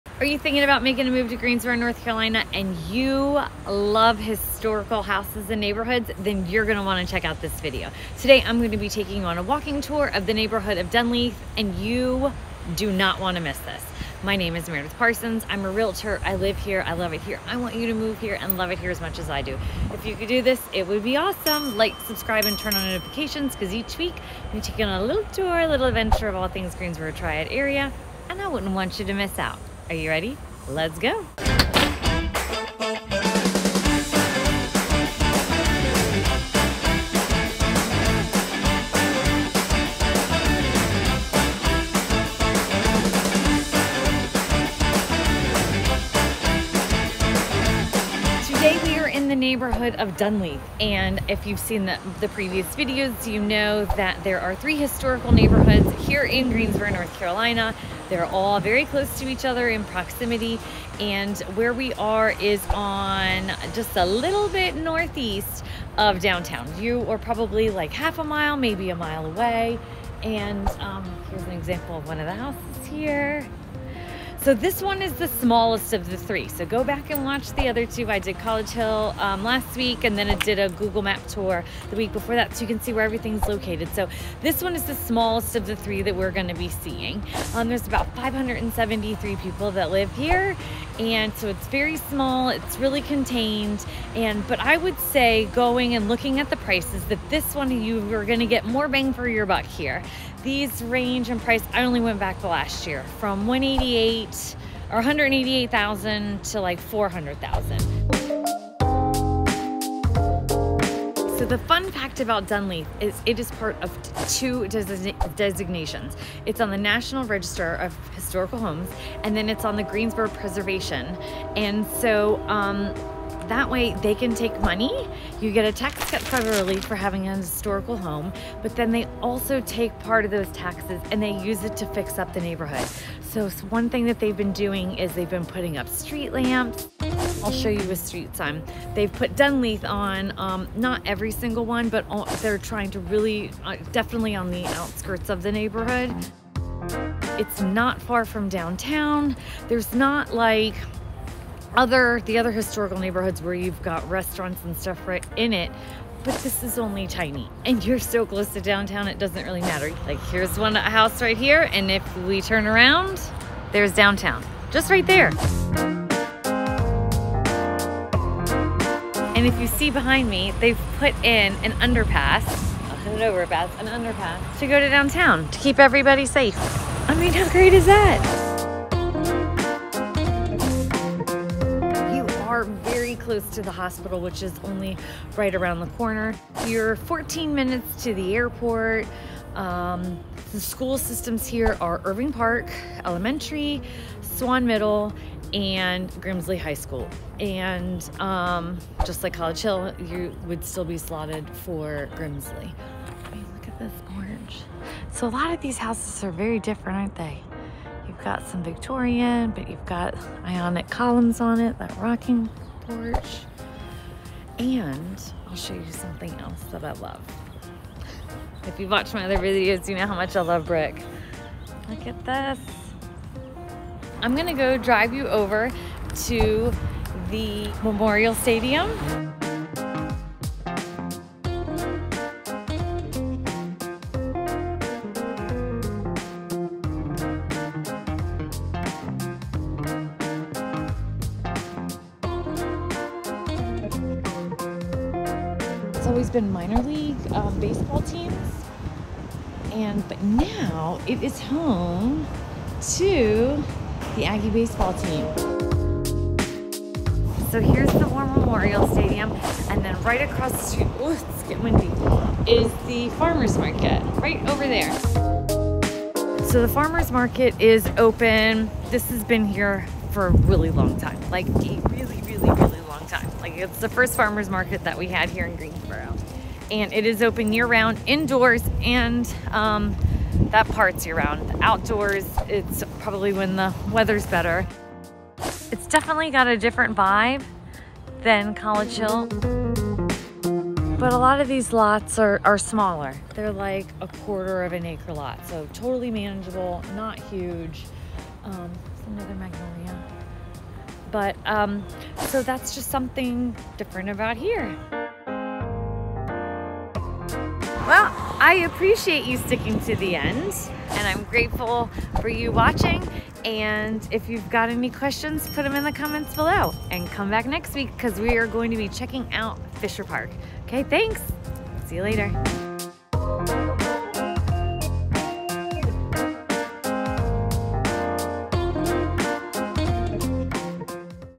Dunleath Historical Neighborhood l Moving to Greensboro North Carolina l Historical Greensboro NC
🏡 Welcome to Dunleath, North Carolina – a quaint neighborhood steeped in charm and history! 🌳 With its tree-lined streets and historic homes, Dunleath offers a serene retreat for residents seeking a peaceful yet vibrant community.

🏡 Welcome to Dunleath: Join us for a walking tour of the historic Dunleath Neighborhood in Greensboro, North Carolina!

🌆 Exploring the Neighborhood: Discover charming streets, stunning views of the Greensboro skyline, and iconic landmarks like War Memorial Stadium.

🏟️ War Memorial Stadium: Learn about the rich history of War Memorial Stadium, a significant landmark in the area.

🍎 Farmers Market: Visit the farmers market, a local treasure dating back to 1874, offering fresh produce and goods year-round.

🏠 Neighborhood Comparisons: Dunleath boasts smaller lots compared to neighboring historical areas like College Hill and Fisher Park.

👨‍👩‍👧‍👦 Community Overview: With 573 residents, Dunleath offers a close-knit community atmosphere.

🛫 Convenient Location: Enjoy easy access to Piedmont Triad Airport, just 14 minutes away, and downtown Greensboro within walking distance.

📚 Schools: Residents are zoned for Irving Park Elementary, Swann Middle, and Grimsley High School.

I'd love to help you make a smooth move to Greensboro North Carolina!

🤔Thinking of moving to Greensboro North Carolina? Let us help!

- google maps
- Triad MLS

⭐️ Pricing subject to change.  Costs, percentages & prices for current market price/value is based on January 2023. ⭐️